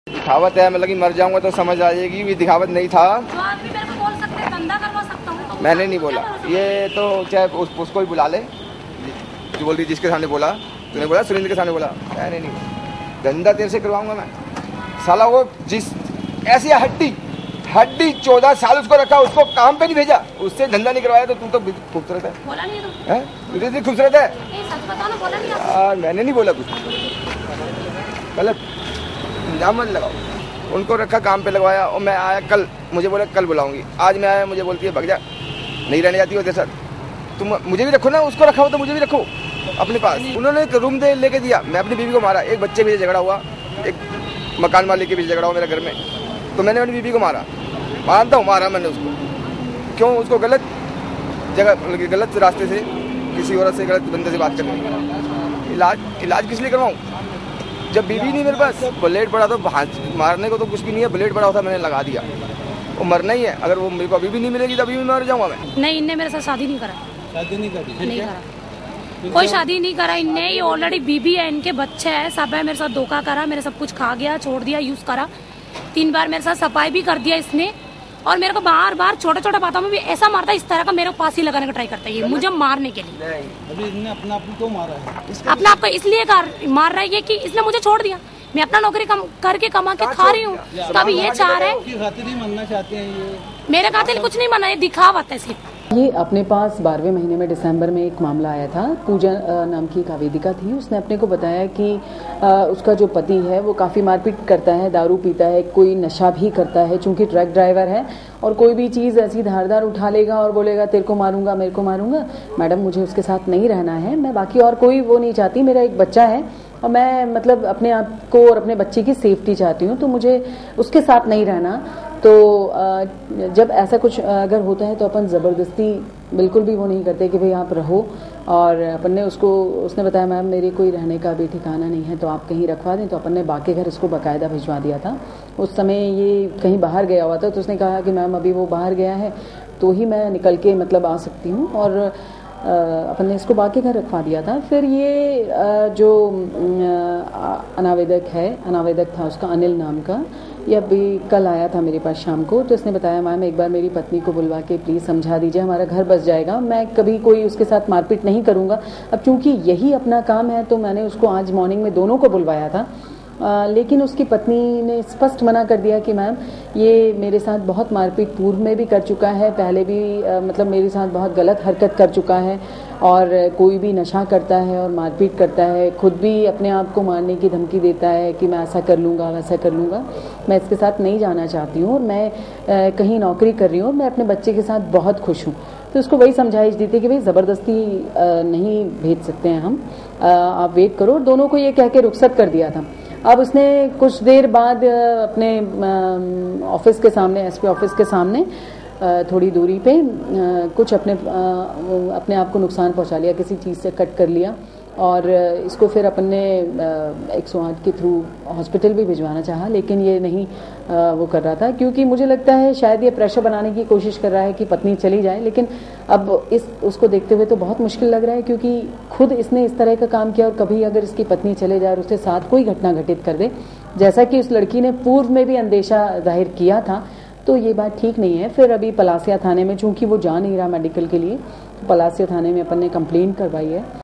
थाने में गला काटकर जान देने की कोशिश
इंदौर। महिला थाने में एक व्यक्ति ने अपना गला काटकर जान देने की कोशिश की। दरअसल, वह अपनी पत्नी को साथ ले जाना था, जबकि वह साथ नहीं जाना चाहती थी। इसी बात से गुस्सा होकर उसने खुद का गला काटकर जान देने का प्रयास किया। काफी देर तक वह थाना परिसर में ही हंगामा करता रहा, लेकिन पुलिस ने उसका इलाज करवाना भी मुनासिब नहीं समझा।